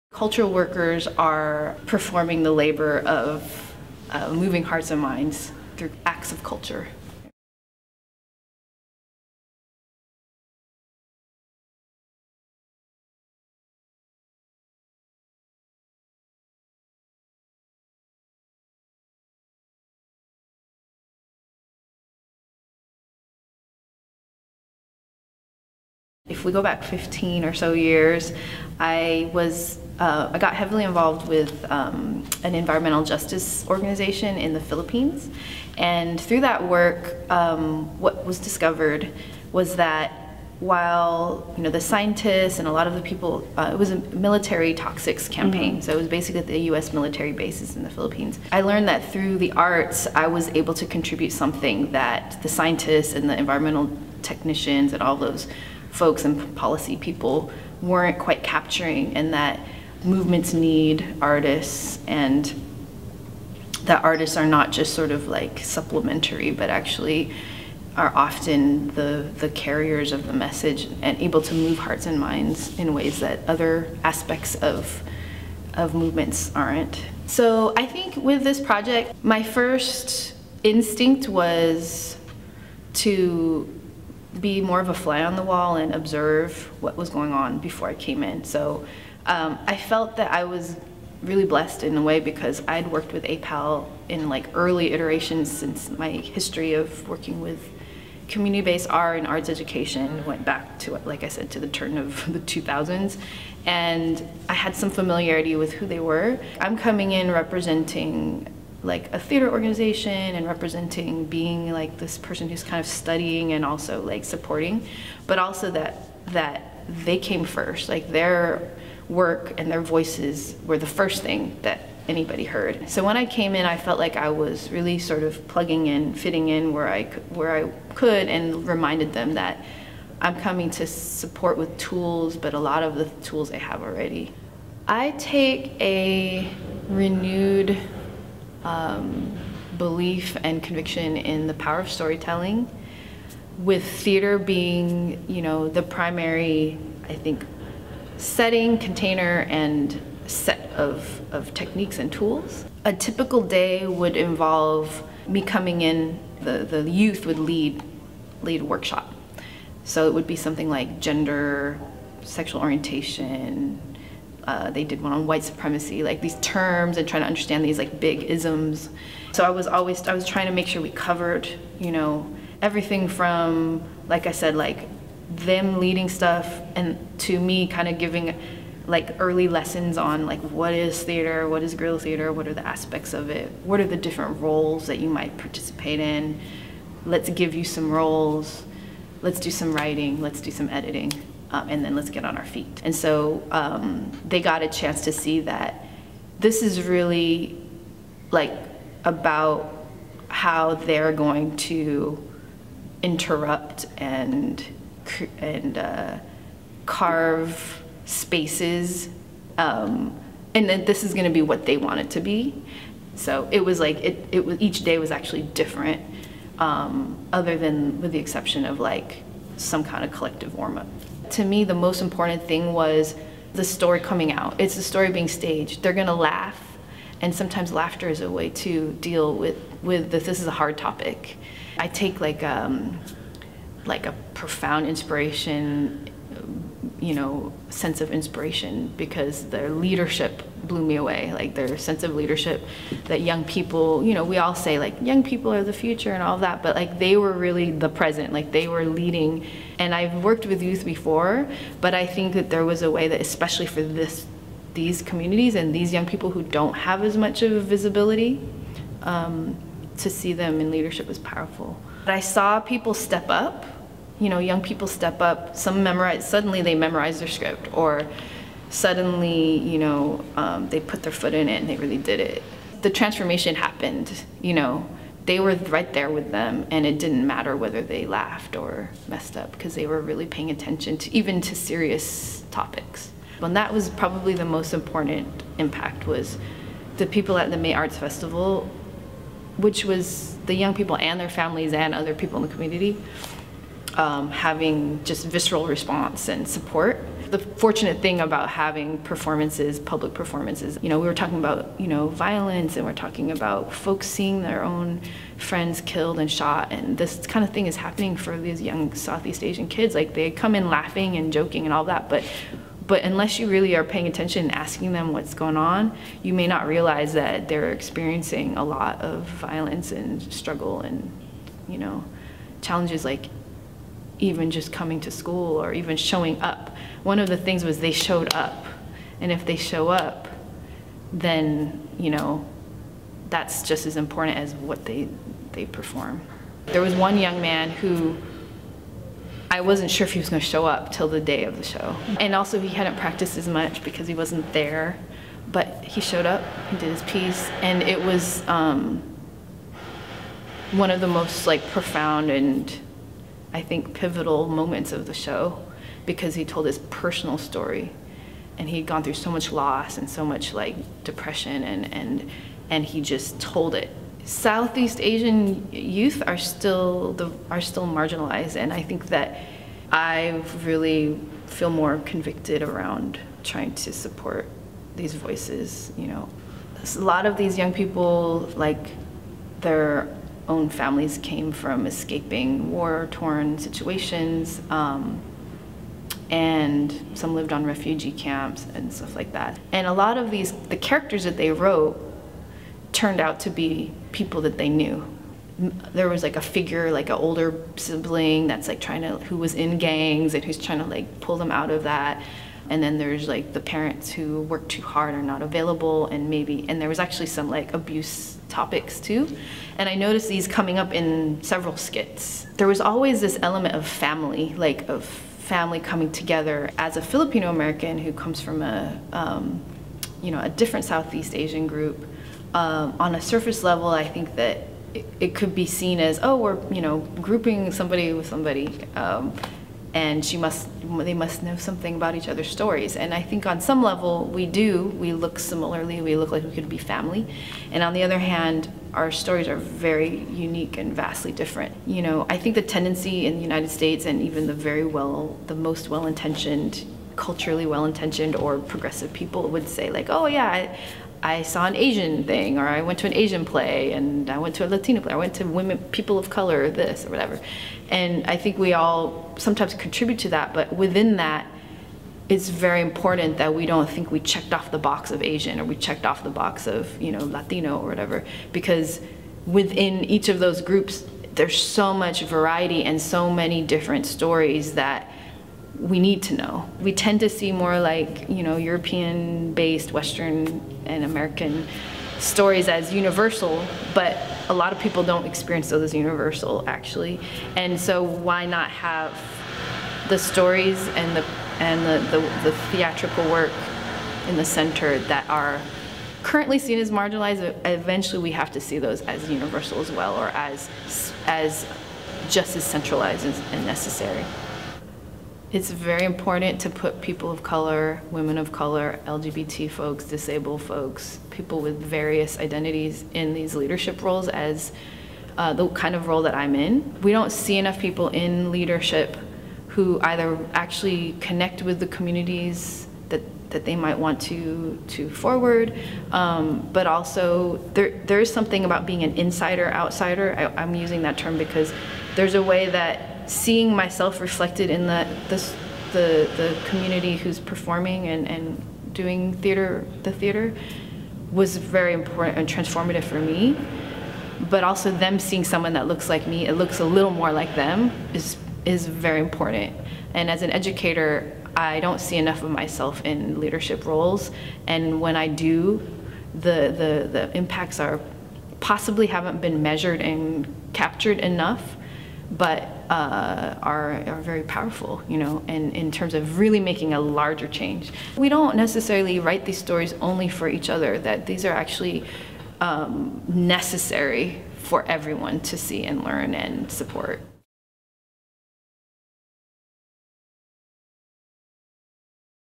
Cal Shakes Artist-Investigator 2016: Aimee Suzara / AYPAL
How can the tools of theater artists be applied outside the rehearsal room? That was the question driving Cal Shakes’ 2016 Artist-Investigator Program. We invited five artists to pair with three nonprofits and collaboratively design participatory art-making projects to advance each group’s mission.

Filipino-American poet, playwright, and performer Aimee Suzara teamed up with AYPAL, working with youth to build and perform guerilla theater addressing the issues most important to them in preparation for AYPAL’s 18th Annual May Arts Festival, a day of cultural resistance against gentrification, racism, and young people’s oppression.

AYPAL is a youth development organization whose mission is to empower Oakland’s low-income Asian & Pacific Islander immigrant and refugee families to be leaders for neighborhood change.